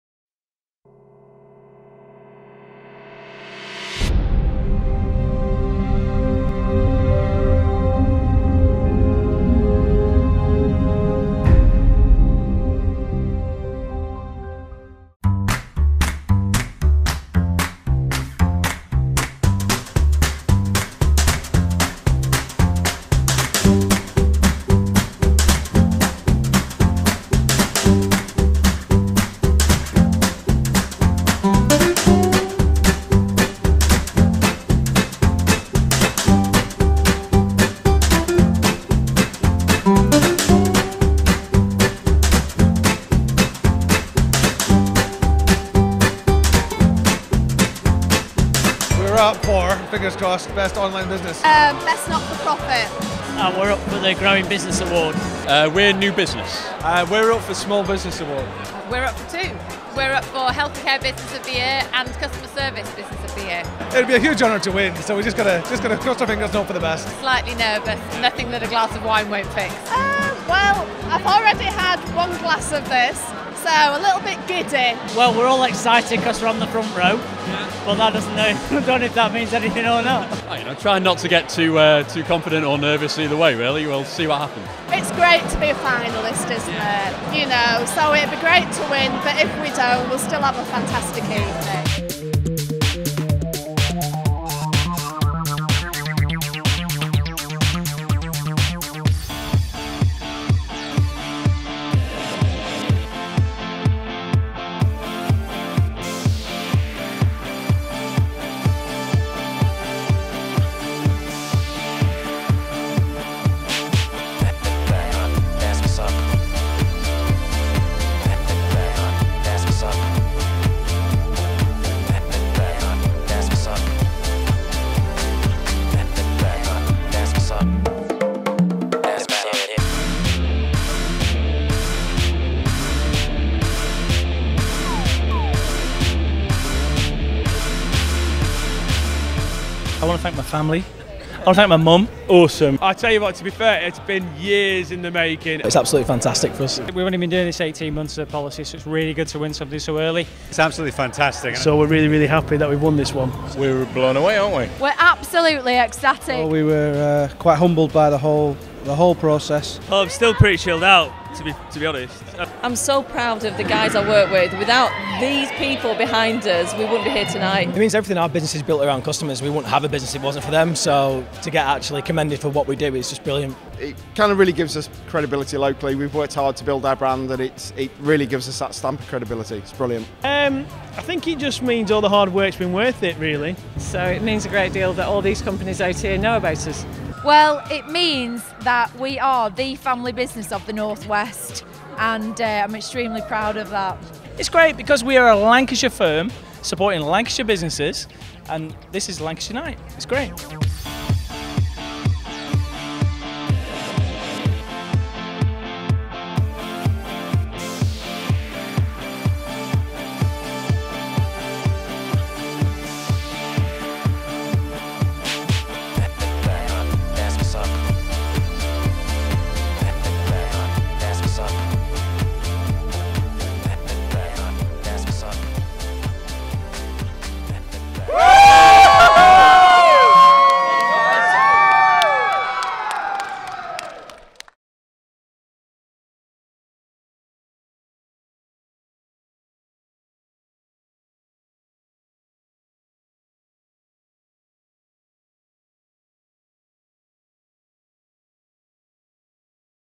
RRA16 highlights video
Highlights of Lancashire Business View's Red Rose Awards 2016 at the Winter Gardens Blackpool